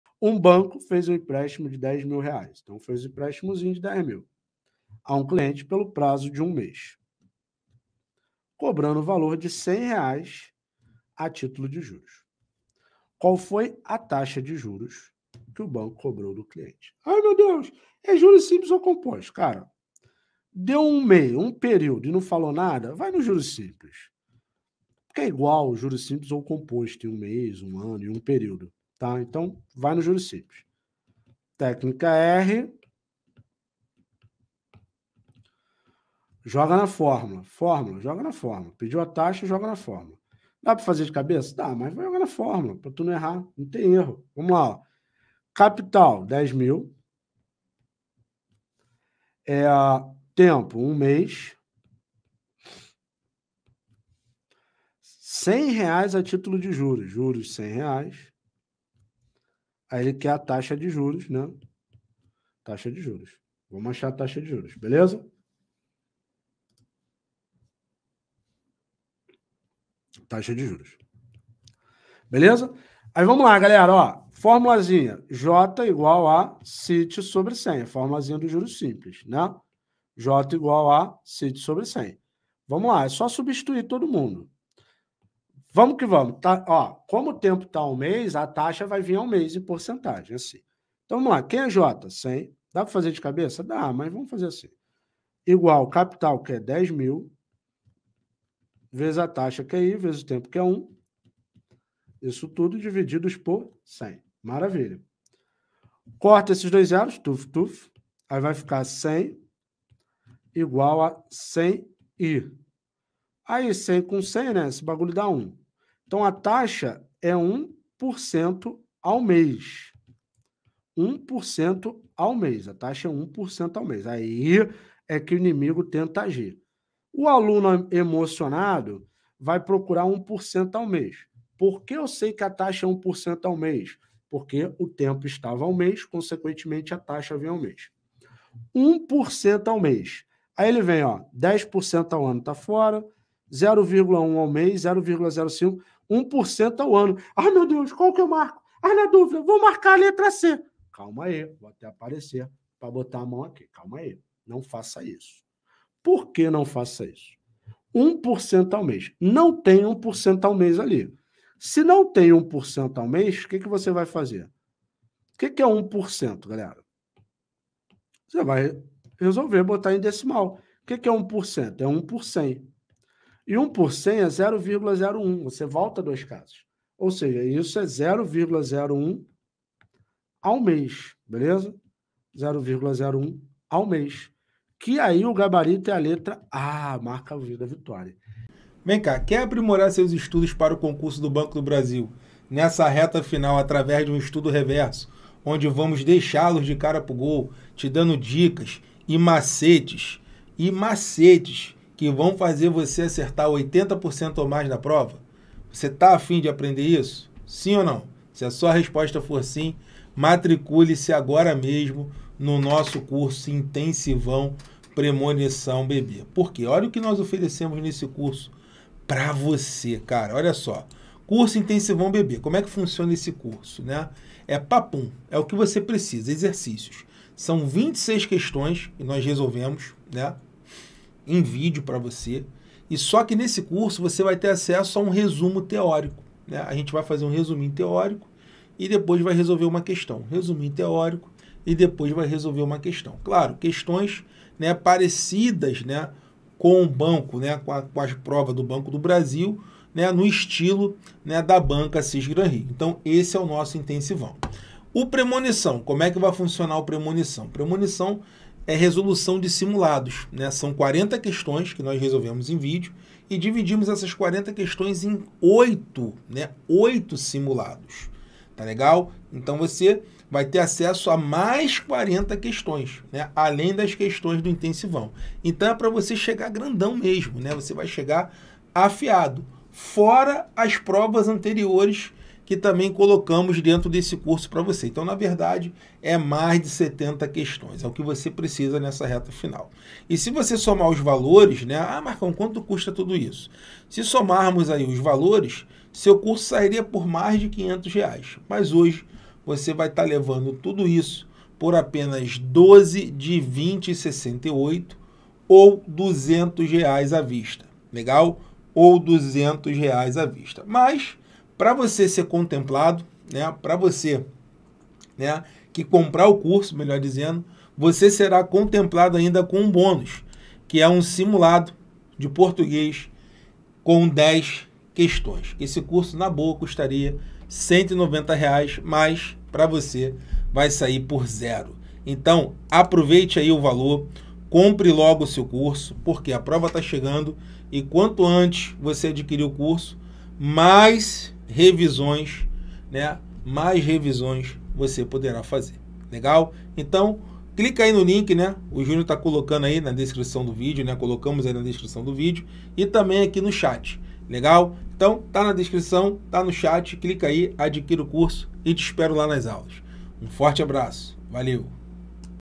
Matemática Financeira para o BB | Juros simples em poucos passos
Você está sofrendo com Matemática Financeira para o Concurso Banco do Brasil?

Então você precisa assistir essa aula onde nosso professor, Marcão, mostra Matemática Financeira com o Método MPP para o Concurso Banco do Brasil de uma forma objetiva e simples.

Você terá um passo a passo para aprender Matemática Financeira e passar no concurso Banco do Brasil 2023 e sair na frente de muitos concorrentes, no último concurso essa matéria foi a que mais reprovou e se você quer passar nesse concurso de agora precisa chegar pronto para Gabaritar.

Agora basta dar o play no vídeo e aprender Matemática financeira para o Concurso do banco do Brasil 2023.

🔴 Correções das provas do Banco do Brasil 2021.

🔴 Playlists para você Aprender Matemática para o Banco do Brasil.